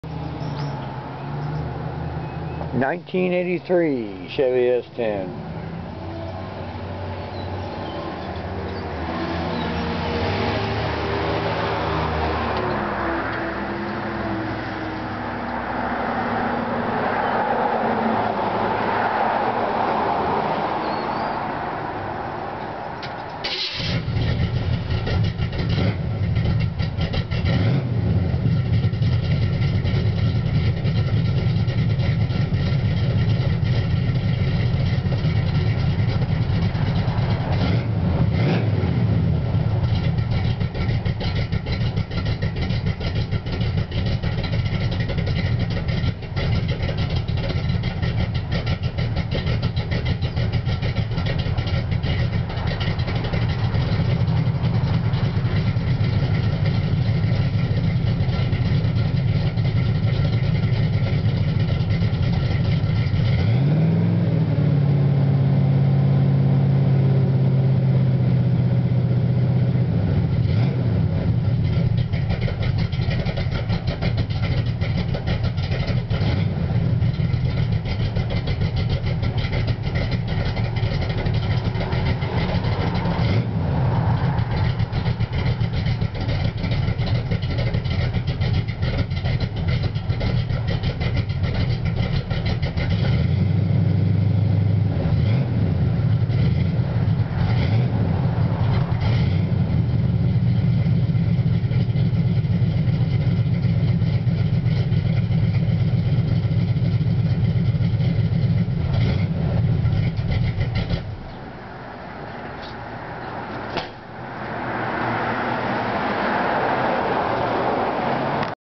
BADASS 1983 Chevy S10 4x4 400,000 Mile Club
This little beauty has over 350,000 miles on her. Run's in the 8's in the 1/8 mile strip. Just got it on March 29th 2009 and after going through everything, we'll test her in the mudpit's at Chillicothe Ohio. 350 engine, 4 bolt main, maxed out with a super racing cam, Hurst shifter (slap stick-old school), 4:10 rearend (posi), and is a beast to drive.